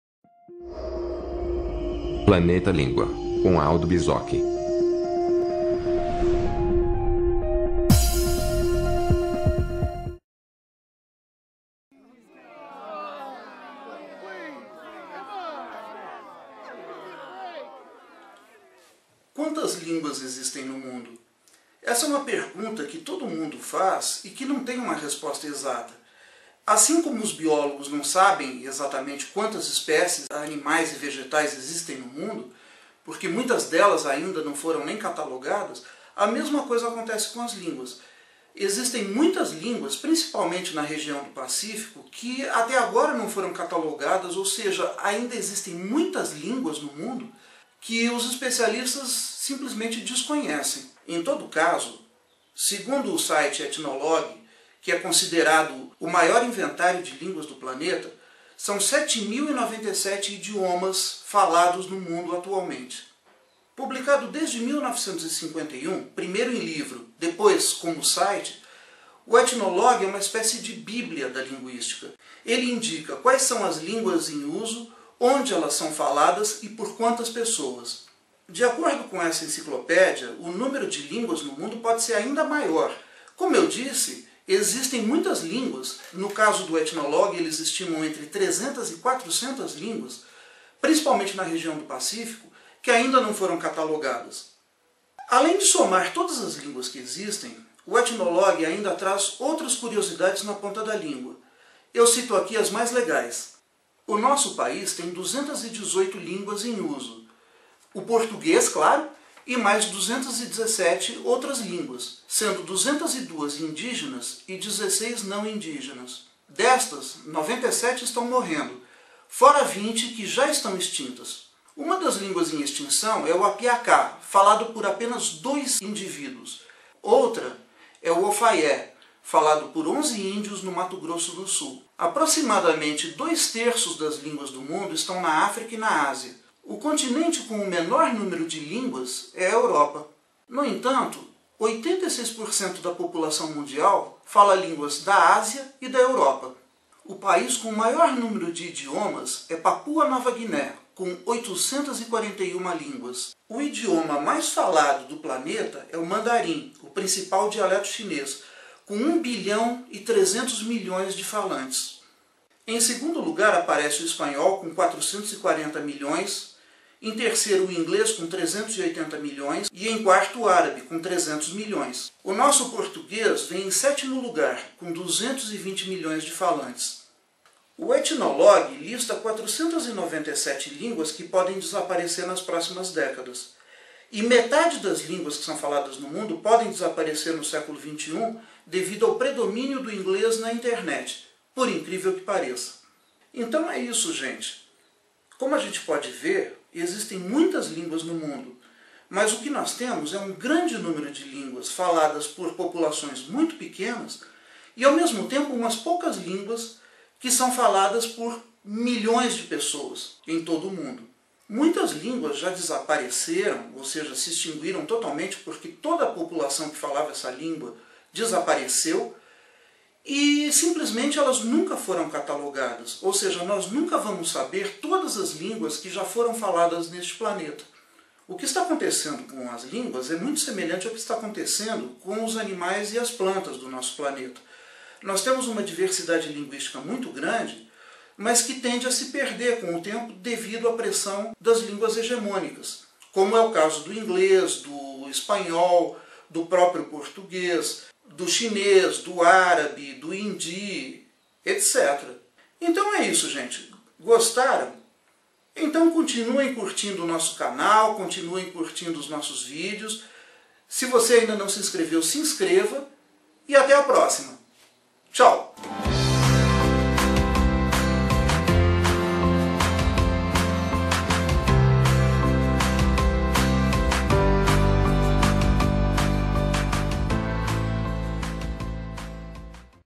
Quantas línguas existem no mundo?
Segundo o site Ethnologue, existem 7.097 línguas no mundo, das quais a grande maioria é falada por muito pouca gente, e algumas poucas são faladas por milhões. Além disso, muitas dessas línguas correm sério perigo de extinção. Na verdade, o número de idiomas pode ser ainda maior, já que muitos deles ainda nem foram catalogados. Neste vídeo, também comento as línguas mais faladas do mundo e os países com o maior número de idiomas.